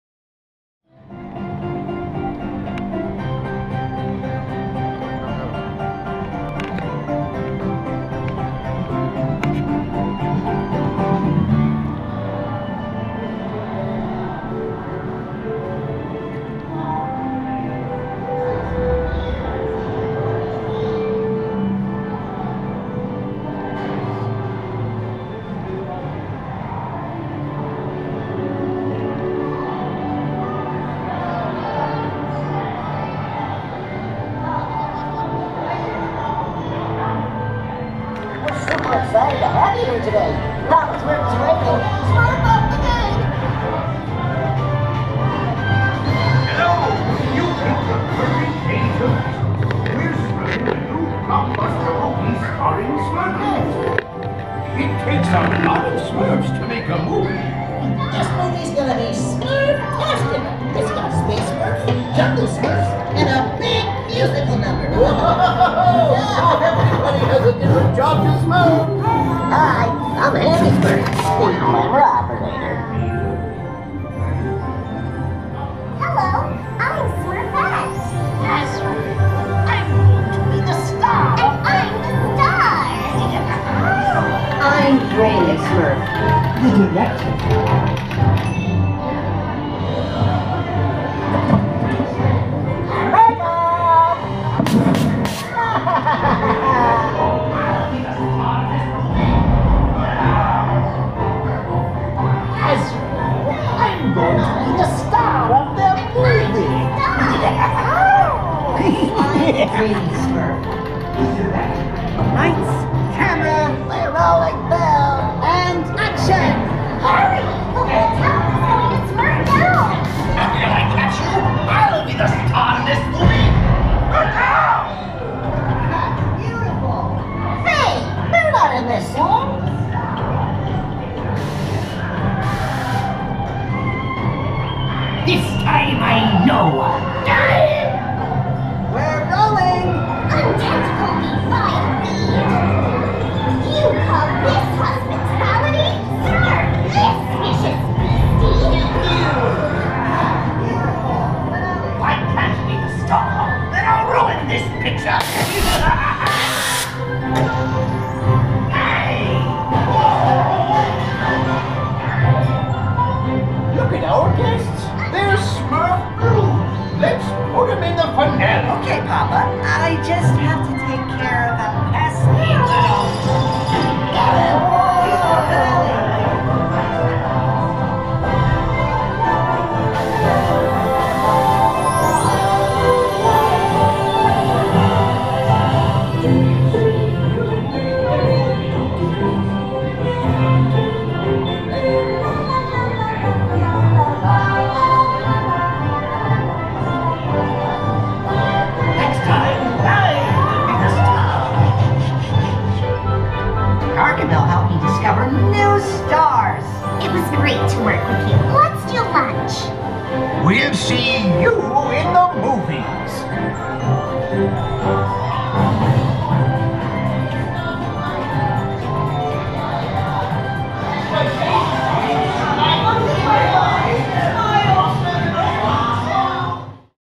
Smurfs studio tour.
Smurf’s Studio tour.  Trackless dark ride.  POV.  Motiongate. Dubai.